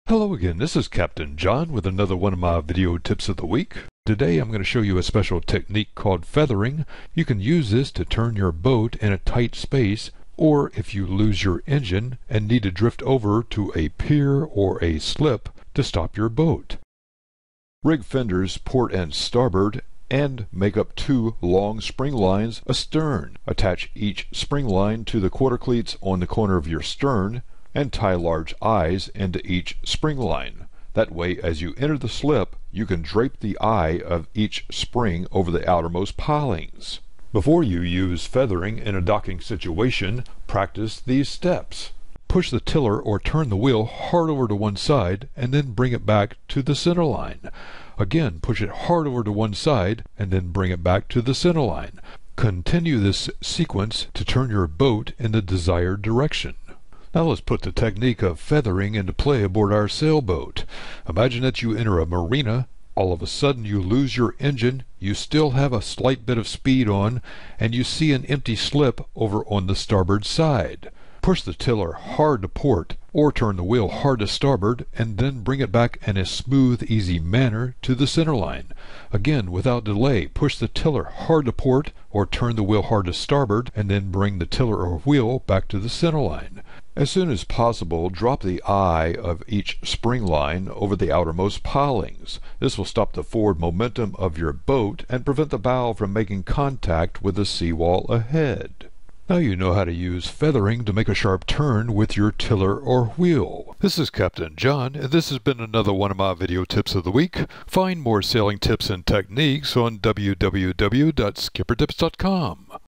How to Dock Your Sailboat with the Magic of Feathering
Imagine you enter a marina when all of a sudden your engine dies. You might have just enough momentum to slide into a slip just off the starboard bow. How can you use your rudder to make this happen? Watch this sailing video to find the answer.